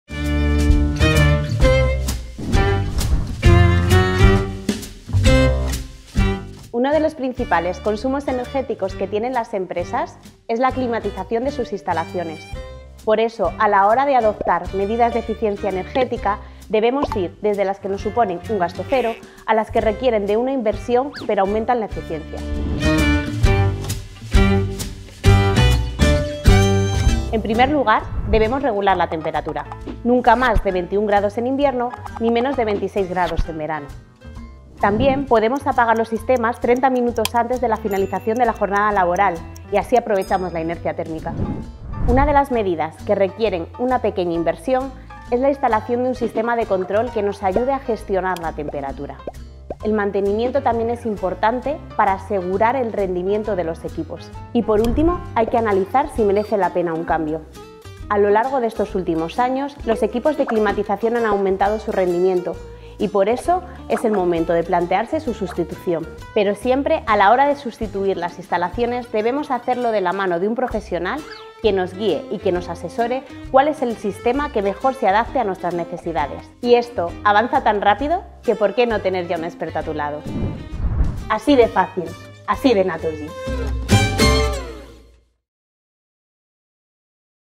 Consejos y soluciones | Climatización
No hay nada como trabajar en un lugar bien climatizado, pero ¿sabes cómo hacerlo de forma eficiente? Te damos los consejos y soluciones que harán de tu empresa la más cálida y eficiente. ¡No te lo pierdas!